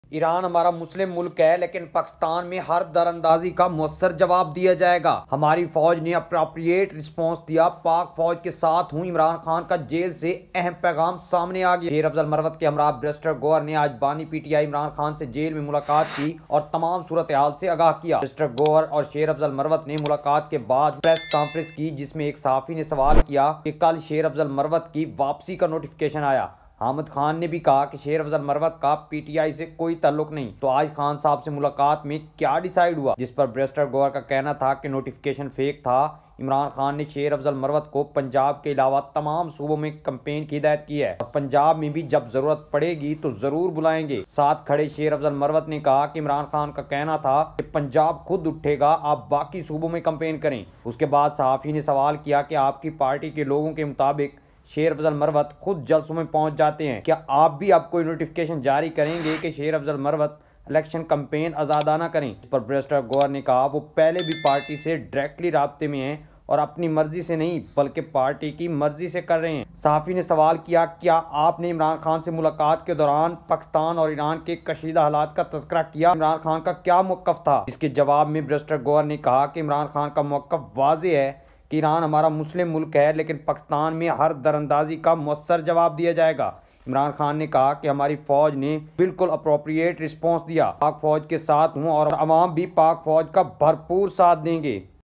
Imran Khan Statement About Pakistan and Iran Issue
Imran Khan gave Big statement from jail about Pakistan and Iran Issue. Imran Khan said that we are woth our pak army. We have proud on our army by such a response.